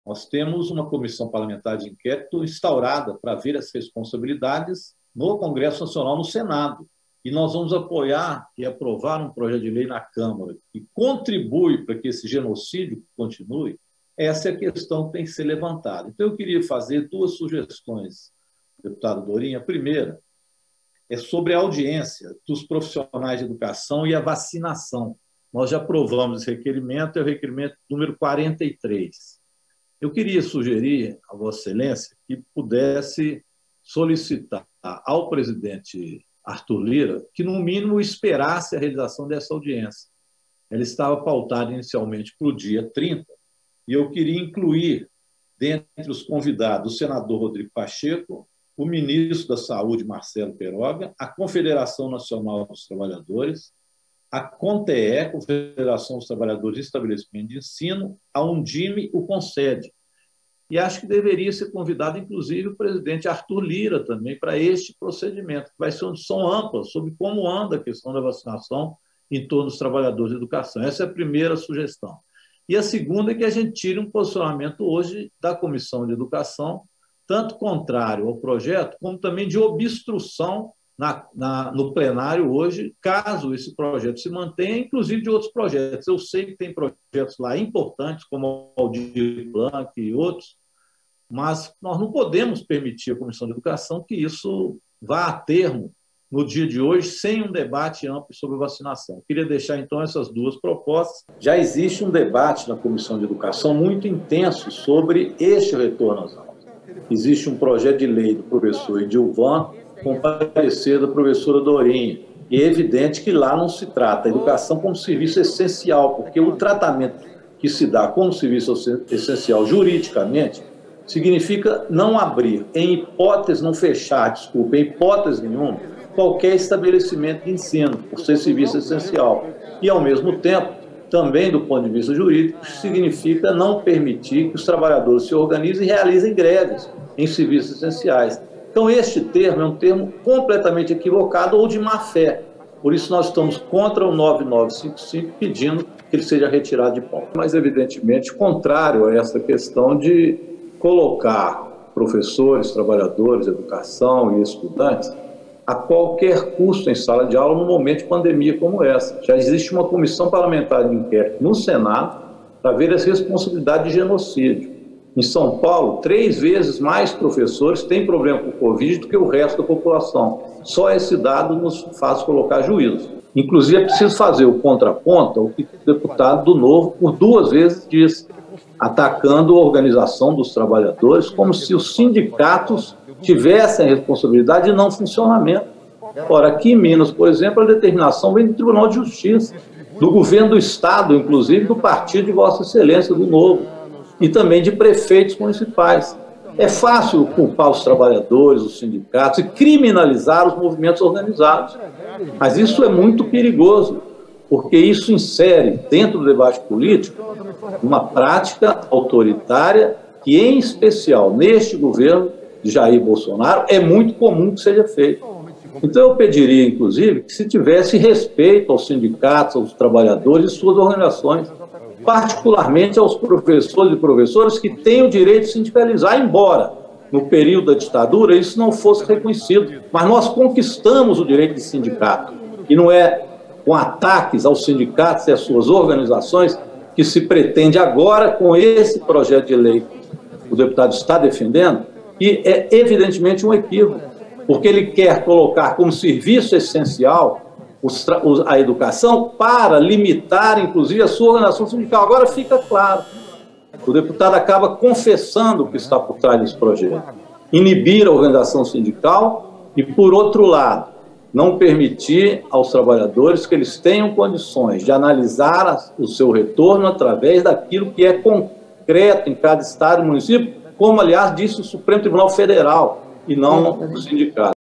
"Defender educação significa defender a vida dos professores!"
“DEFENDER EDUCAÇÃO SIGNIFICA DEFENDER A VIDA DOS PROFESSORES”. NÃO AO PL 5595!

A volta às aulas presenciais no pior momento da pandemia, como defendem a bancada dos grandes grupos privados de ensino, é desrespeitar um dos elos mais importantes do processo educacional: as professoras, os professores e os trabalhadores em geral da Educação. O deputado federal Rogério Correia (PT-MG) ocupou várias tribunas virtuais da Câmara na manhã desta quinta 15 de abril para criticar a proposta de retorno às aulas presenciais neste momento. Ele falou na sessão do Parlamento e na Comissão de Educação, da qual é um dos membros. Veja neste vídeo de pouco mais de 4min um resumo das falas. Por favor, compartilhe para chegar a mais pessoas. Obrigado!